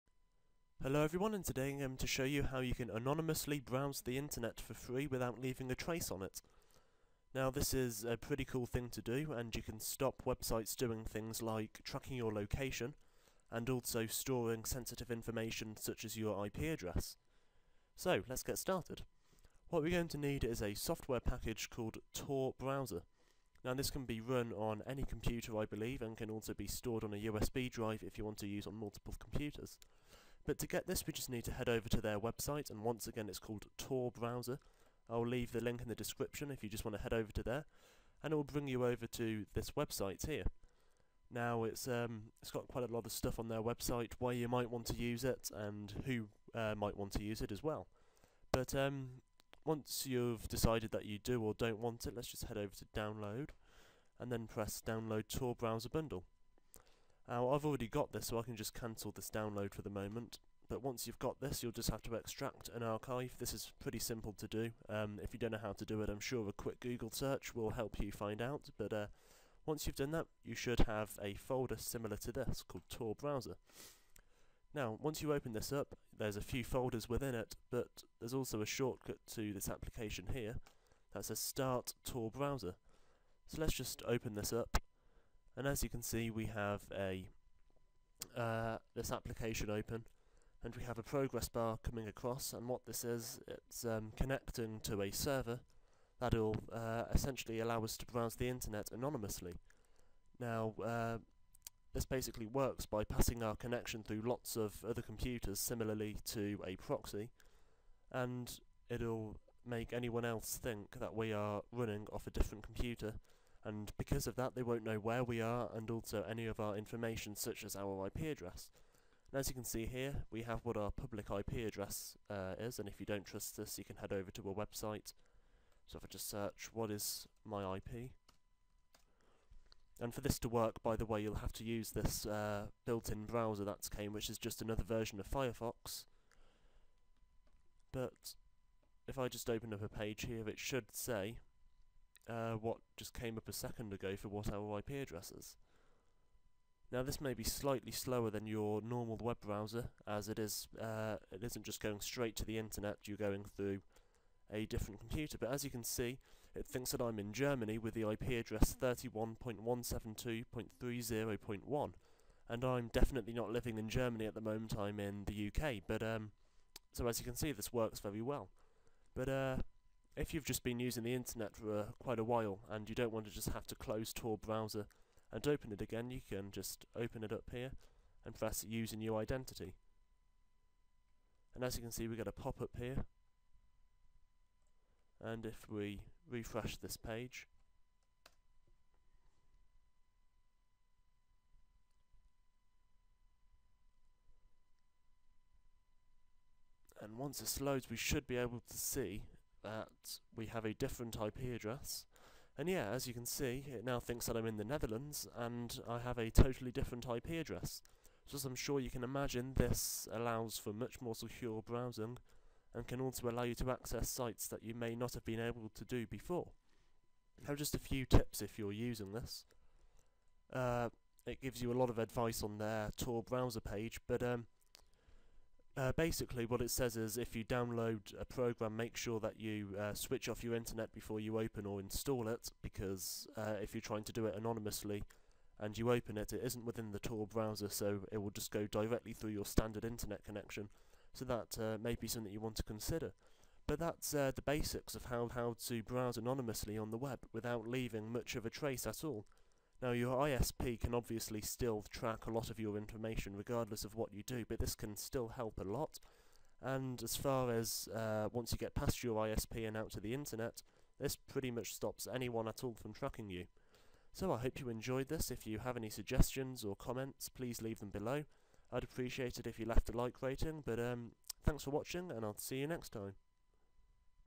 How to Browse the Internet Securely
Today I am going to show you how to securely browse the internet so that any websites you visit don't have any record of you or your IP, and therefore location.